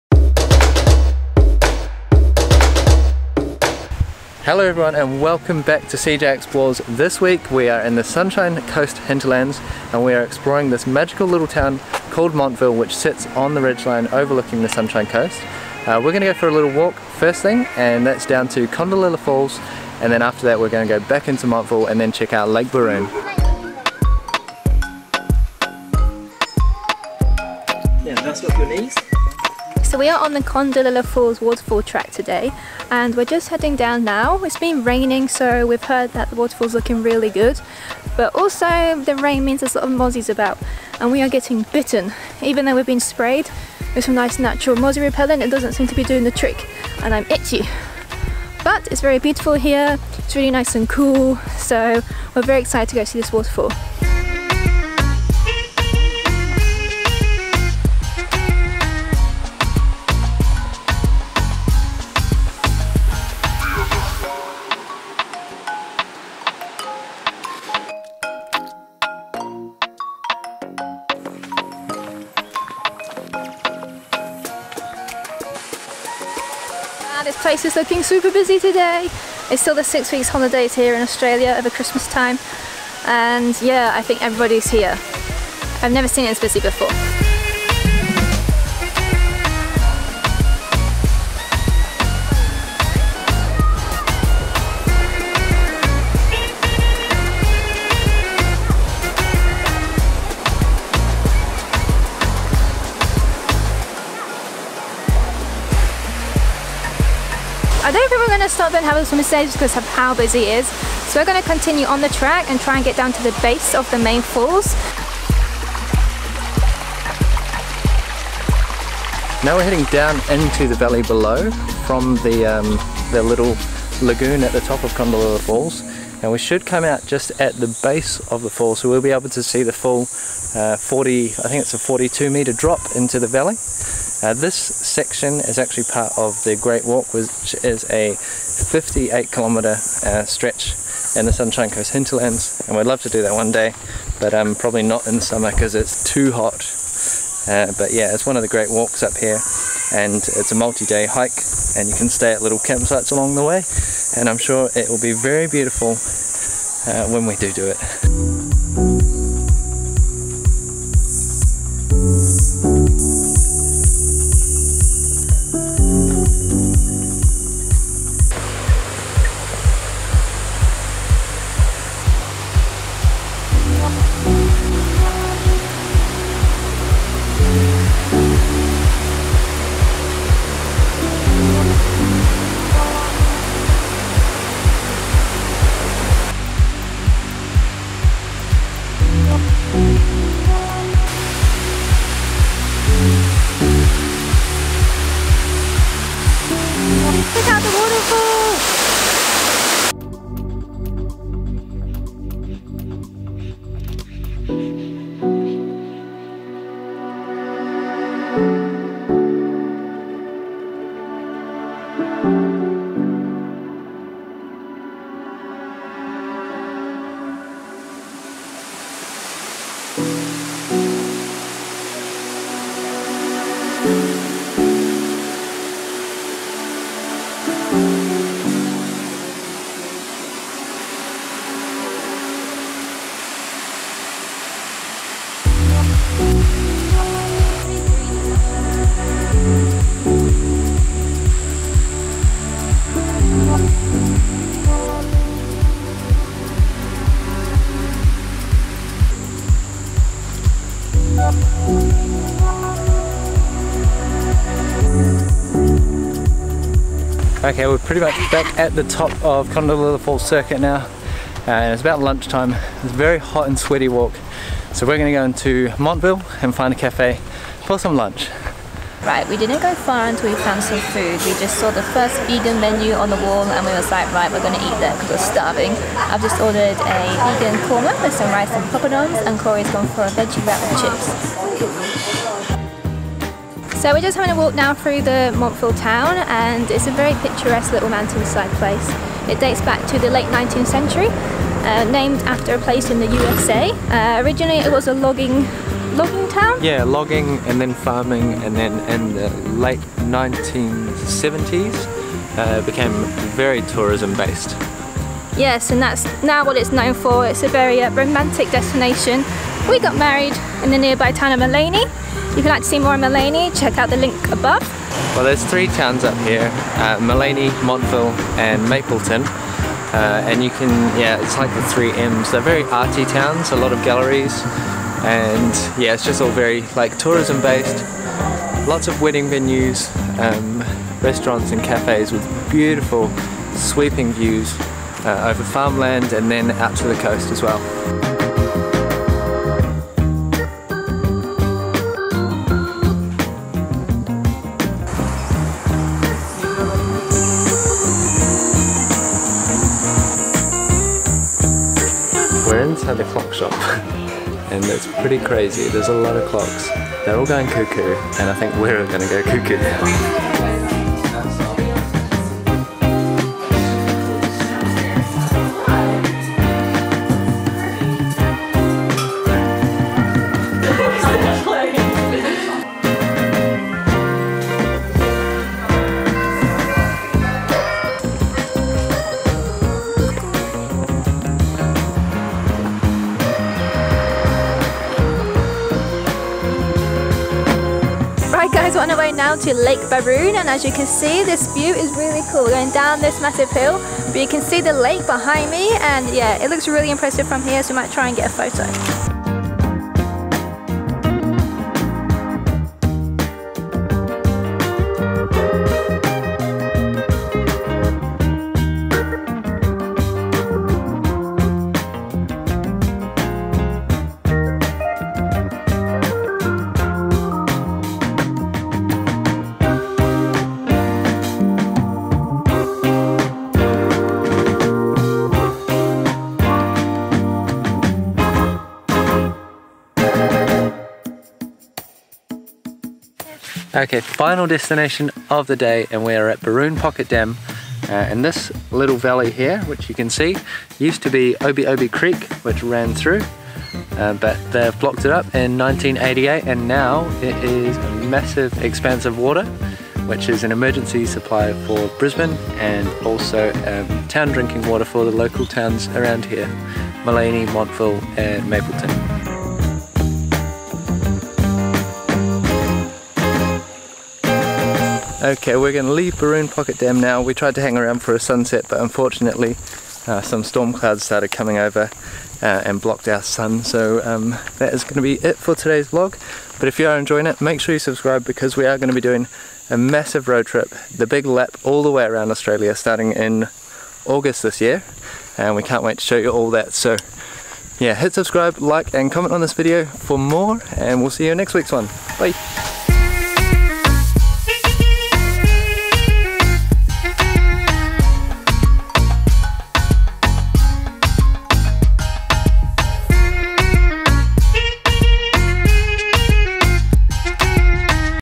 What To Do: Montville, Sunshine Coast Hinterland | Kondalilla Falls, Baroon Pocket Dam
Join us as we explore the mountain town of Montville in the Sunshine Coast Hinterland, Queensland. Our day starts at Kondalilla Falls, where we walk the 4.7km circuit to the base of the falls. Afterwards, we head to Montville town to grab some lunch and explore the stores nestled amongst the rainforest. To end of our day we drive to the nearby Baroon Pocket Dam.